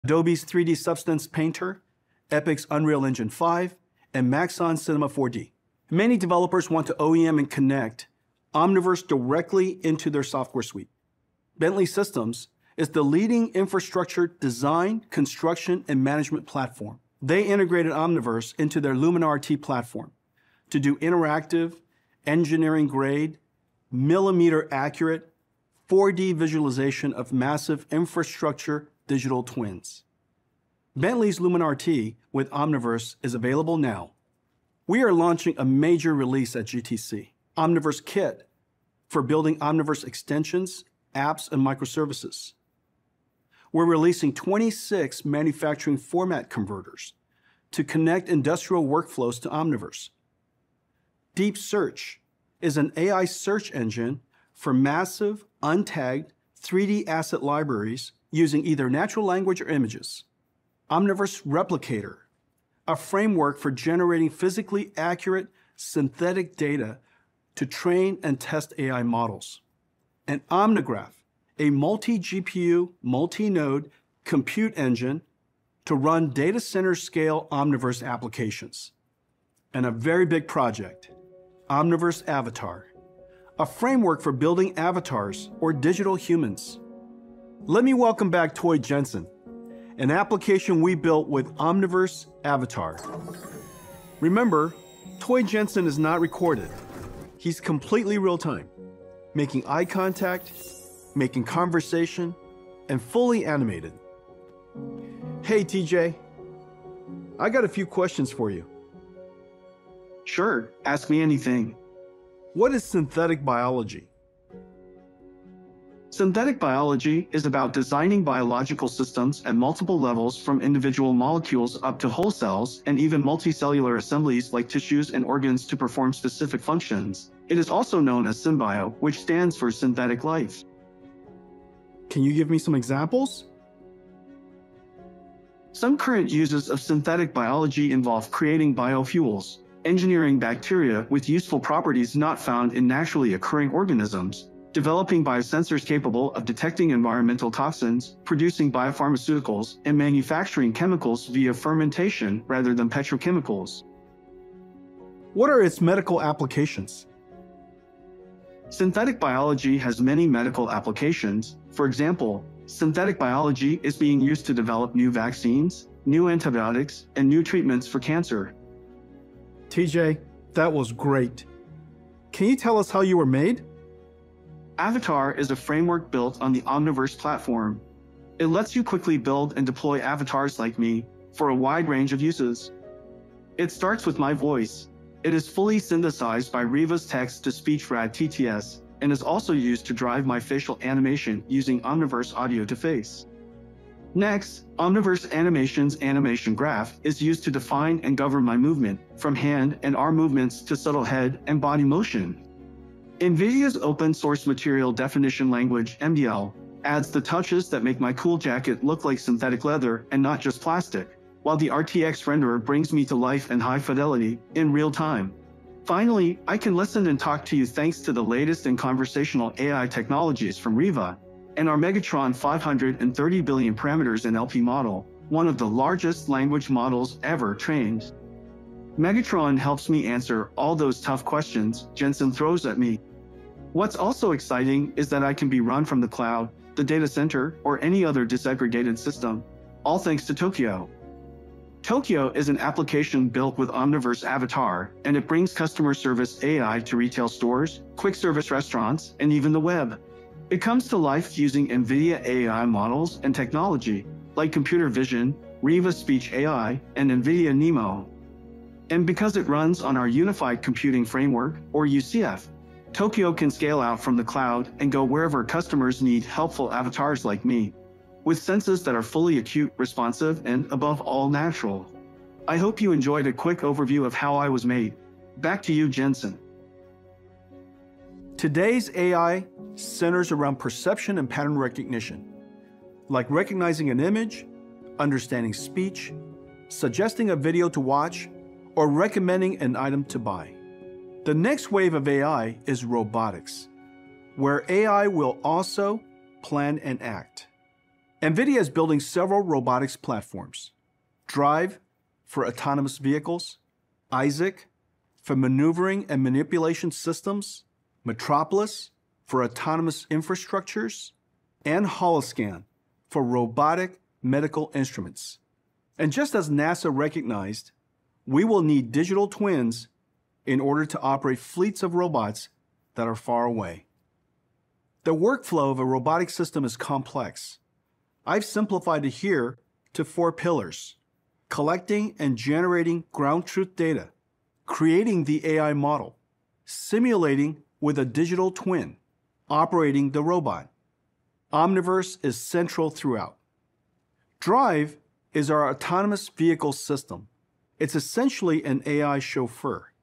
Watch Nvidia Demo Toy Jensen Omniverse Avatar (GTC '22)
At Nvidia's spring GTC, the company's CEO, Jensen Huang, demos its latest version of its Omniverse avatar technology.